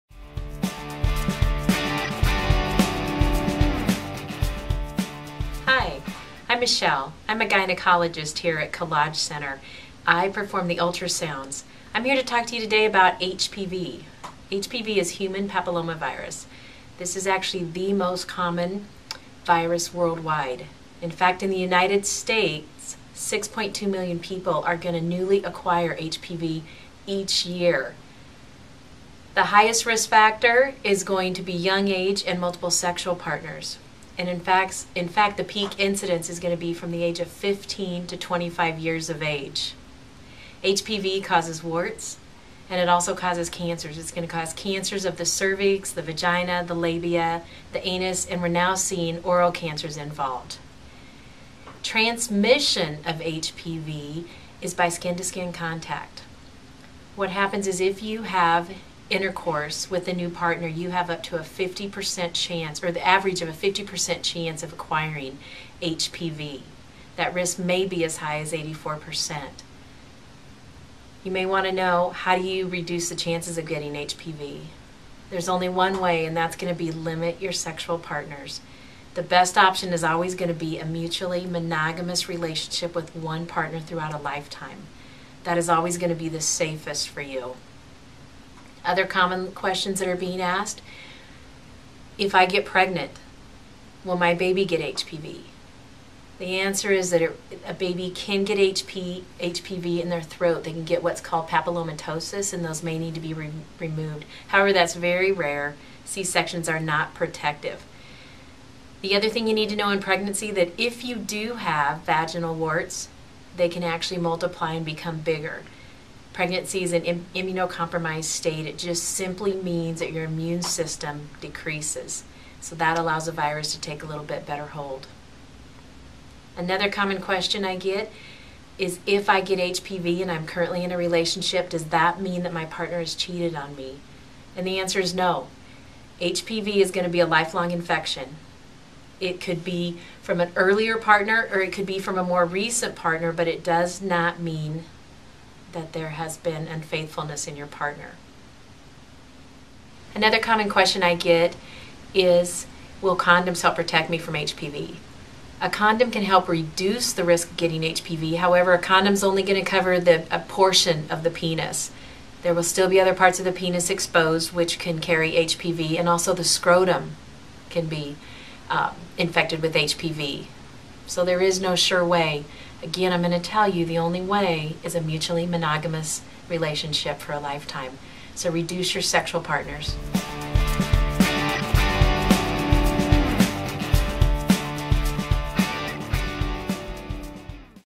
HPV with Dr. Michele
How do I get HPV? How can I reduce the risk of getting HPV? If I get pregnant, will my baby get HPV? If I am in a monogamous relationship and my partner is diagnosed with HPV, does that mean that he/she has cheated on me? Will condoms help protect against HPV?

All of these questions will be addressed by Dr. Michele who performs ultrasounds at Collage Center in Kearney, NE. Want to know more about Collage Center?